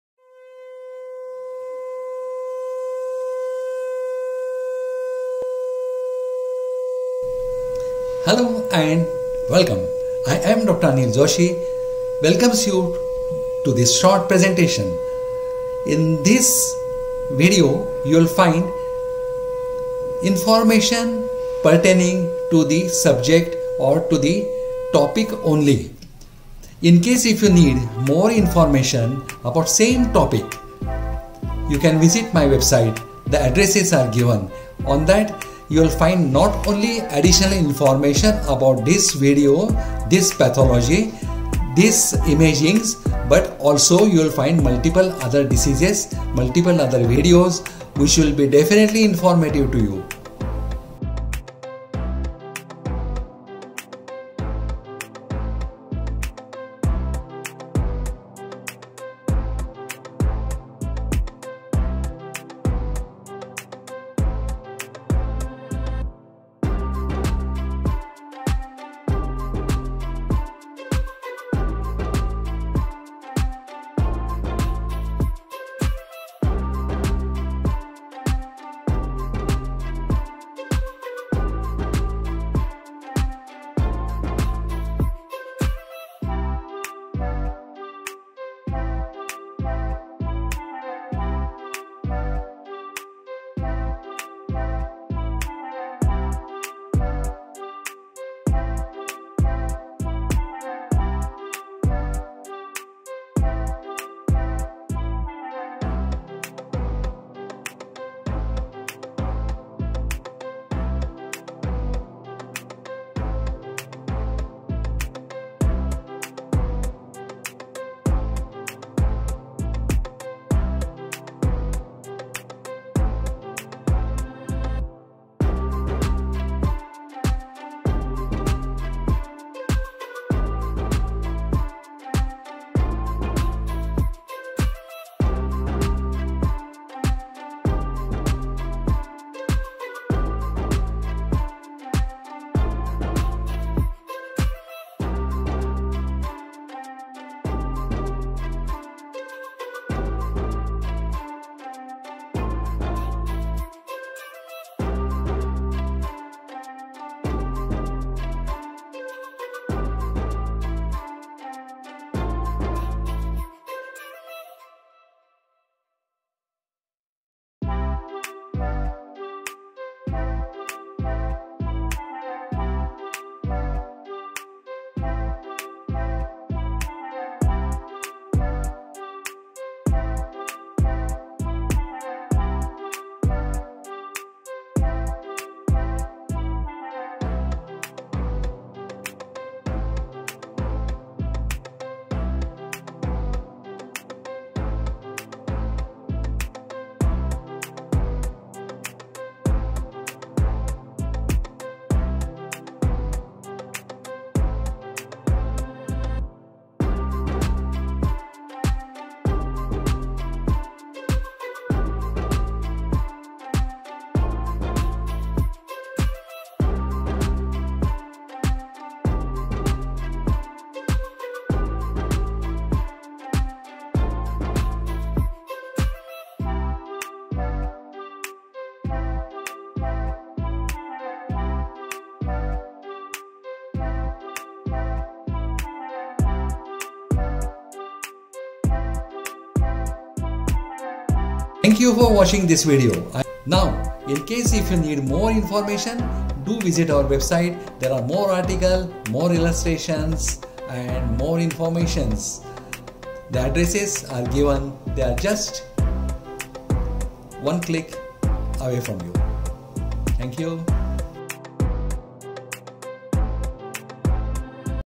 IMAGING PATTERNS OF FIBROCYSTIC DISEASE IN MAMMOGRAPHY & DOPPLER SONOGRAPHY - BY DR. ANIL JOSHI.
MAMMOGRAPHY PLAYS AN IMPORTANT ROLE IN THE DETECTION OF DIFFERENT BREAST DISEASES. IT IS TRUE THAT ONLY FEW OF THESE ARE MALIGANANT  DISEASES. THIS LECTURE IS ABOUT IMAGING PATTERNS OF FIBRO-CYSTIC DISEASES- IN THIS IMAGING GALLARY WE HAVE GIVEN DETAILS OF DOPPLER SONGRAPHIC & MAMMOGRAPHIC APPEARANCES.. - IT IS A PROJECT BY LEARN RADIOLOGY WITH DR, ANIL JOSHI SERIES .